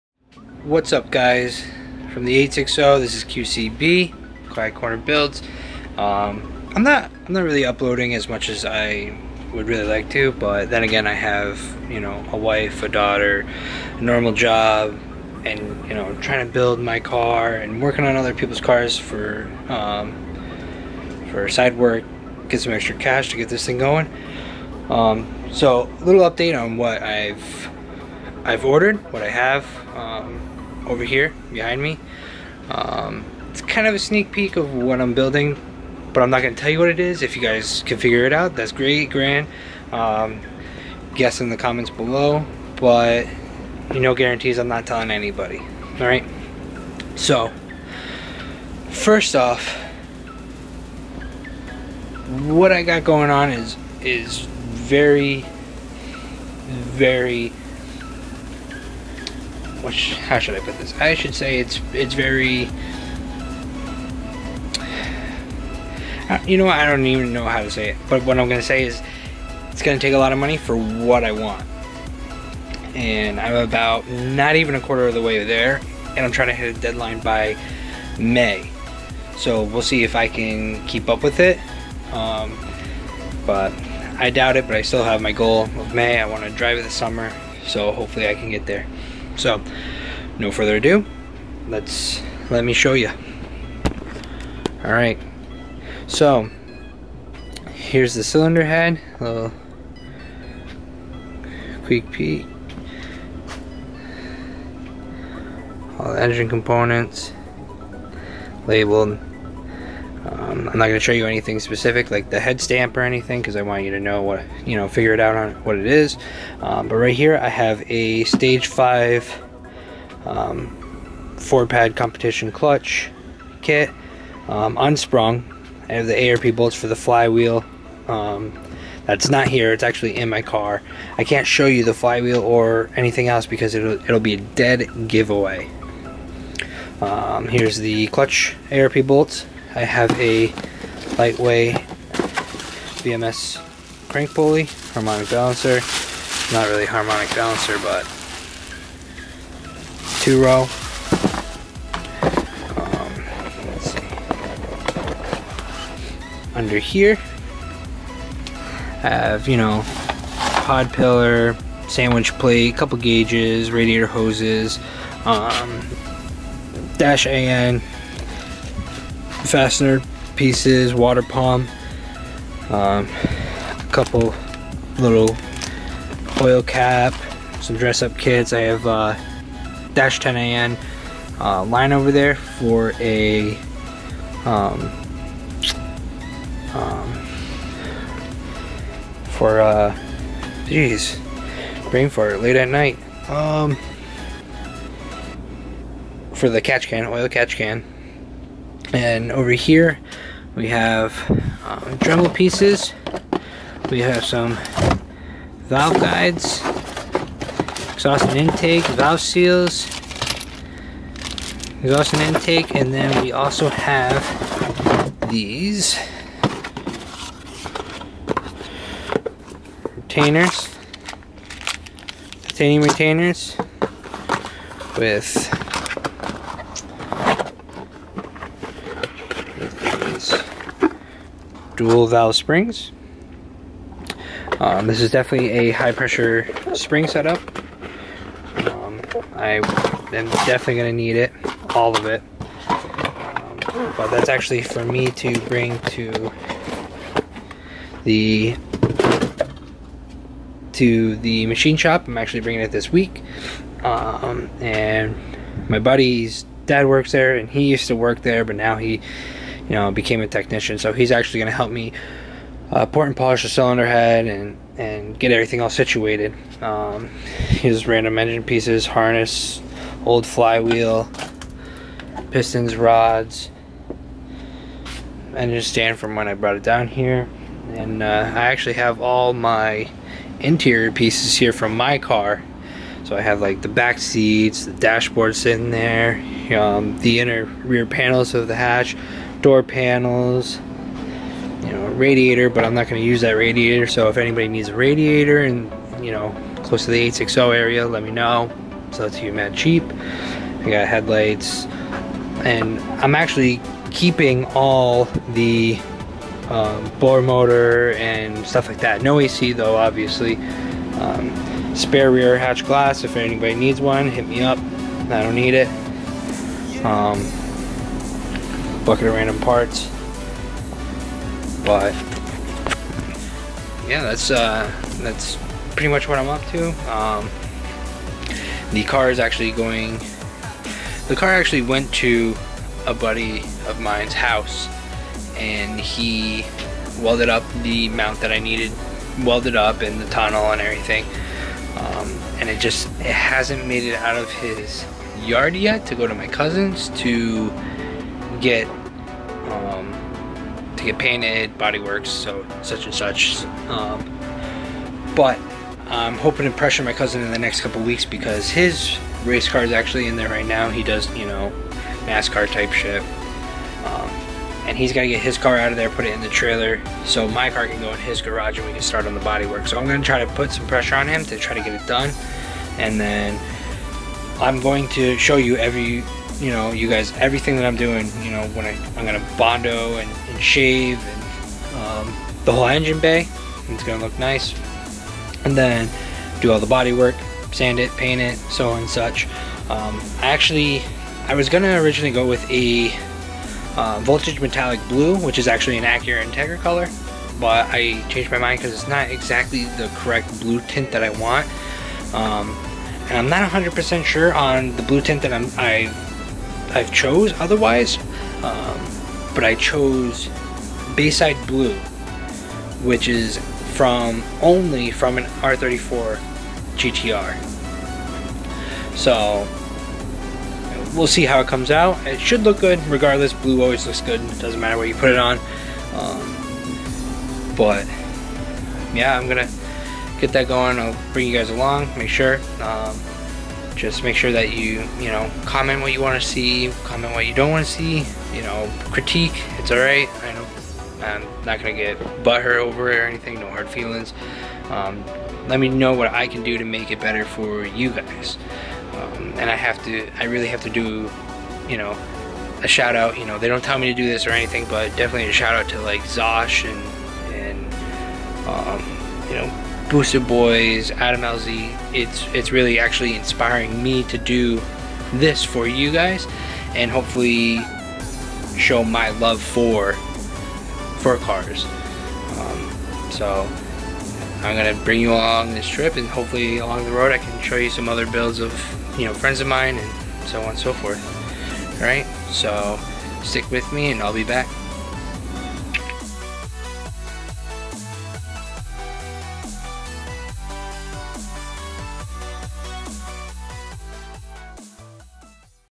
Update on build
just a brief update on my build and some of the new go fats parts that come in.